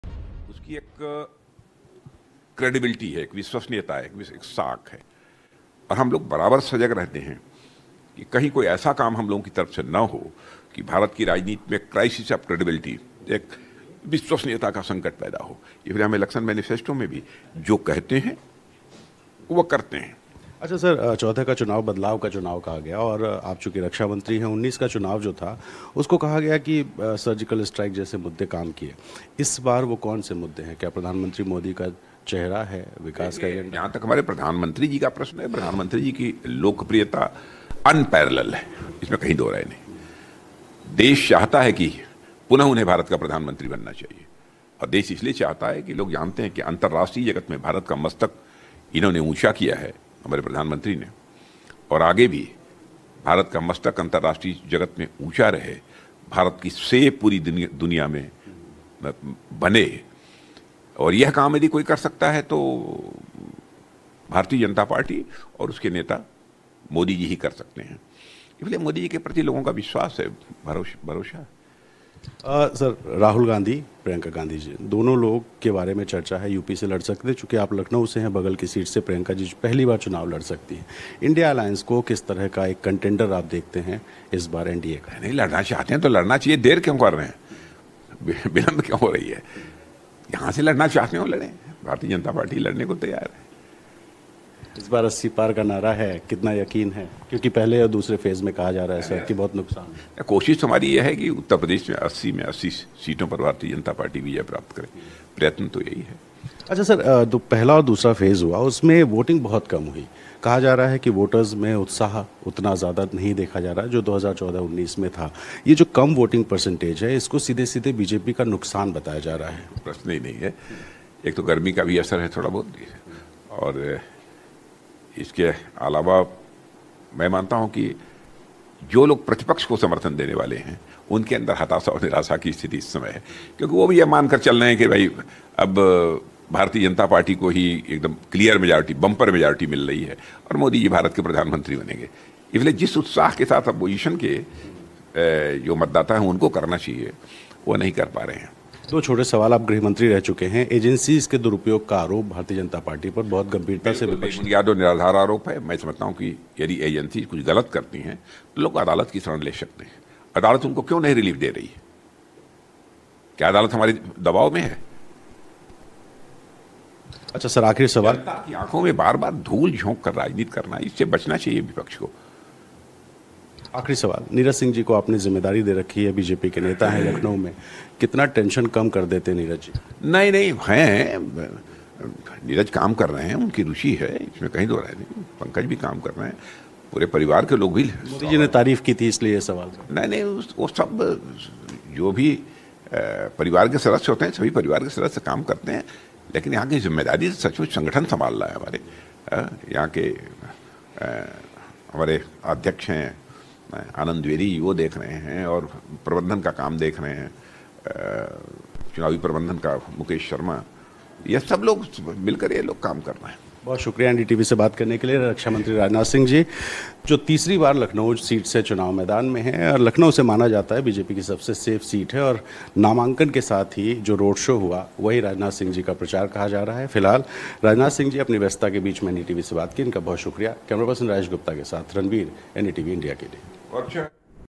Lok Sabha Elections | Rajnath Singh On Buzz Over Rahul, Priyanka Gandhi Contest: "Ready For Fight"
Defence Minister Rajnath Singh expressed confidence today that the BJP would score 80/80 in Uttar Pradesh, shrugging off any concern that low voting could be bad news for his party. Rajnath Singh, who is the BJP candidate from Lucknow, also slammed what he called the opposition's 'politics of obfuscation' in an interview with NDTV on the campaign trail. "PM (Narendra Modi)'s popularity is unparalleled. He has raised India's stature in the world. The country wants him to the Prime Minister. People trust Modi," the Defence Minister said.